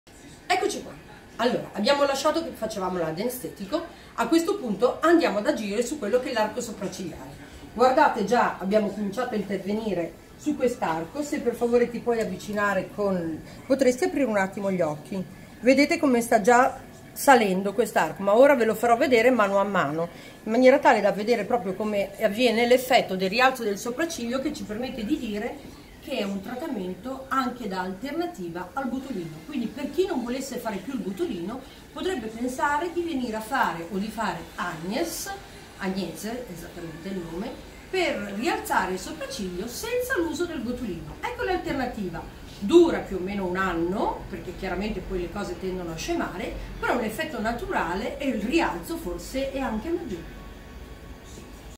TRATTAMENTO PALPEBRE SUPERIORI DEGLI OCCHI CON RADIOFREQUENZA AGNES
Trattamento di rialzo del sopracciglio con radiofrequenza monopolare a micro aghi Agnes di Agnes Medical: valida alternativa al botulino per ringiovanire la zona perioculare e rialzare le sopracciglia in modo naturale e non invasivo!
E' necessaria una seduta per ottenere un miglioramento del 30 - 40 % rispetto alla situazione di partenza!